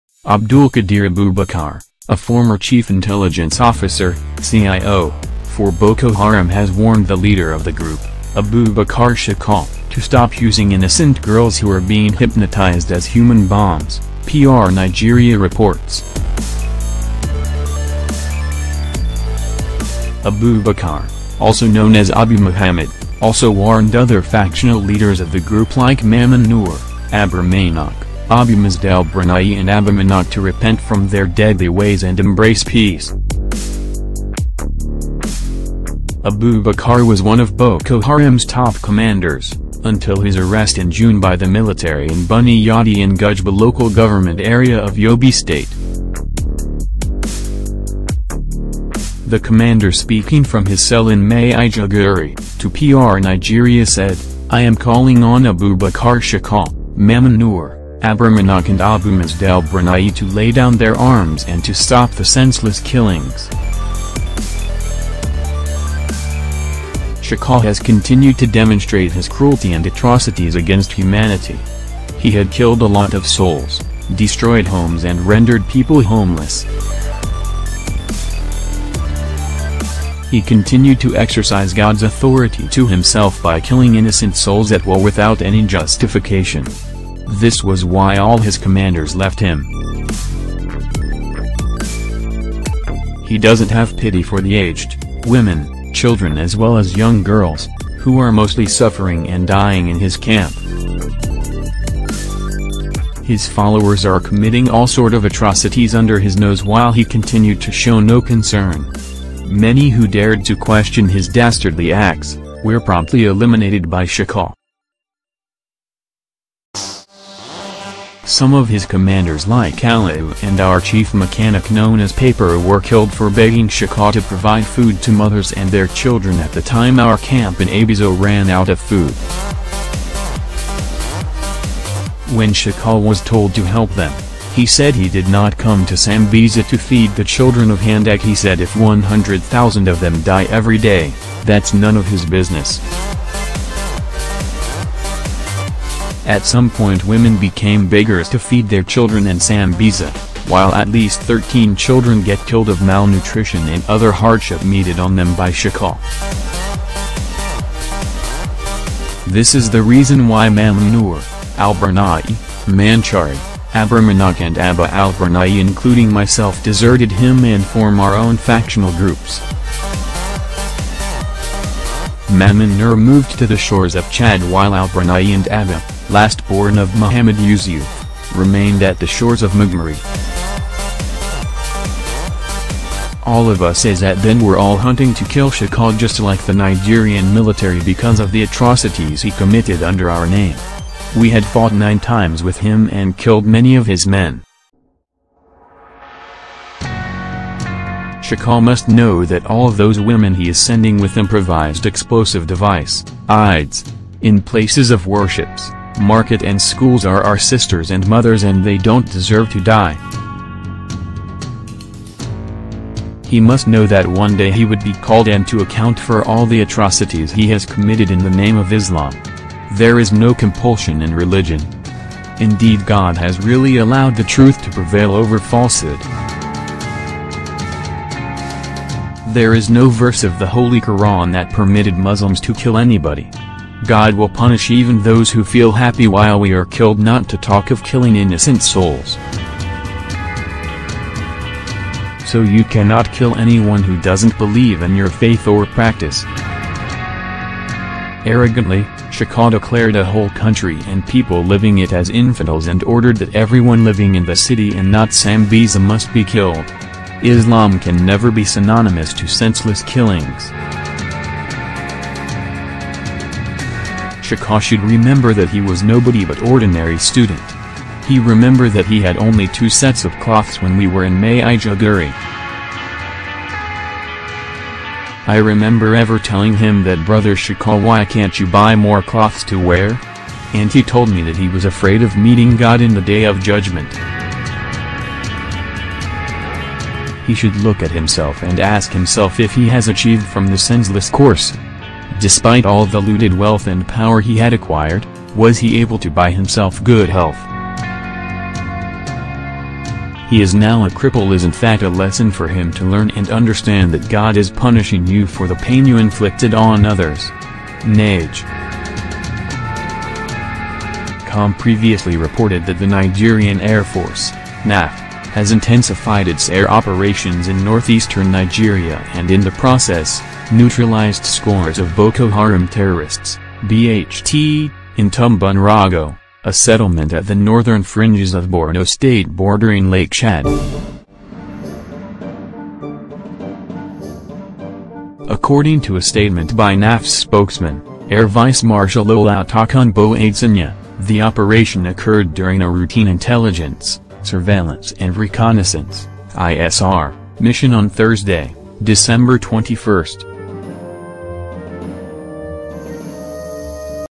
Lay down your arms and stop killing - Former Boko Haram intelligence chief officer
- Abdulkadir Abubakar, former Boko Haram Chief Intelligence Officer (CIO) opens up on the leader of the insurgents, Abubakar Shekau

- Abubakar was a top commander, until his arrest by the military in June

- He also warns other factional leaders of the group to repent from their ways and embrace peace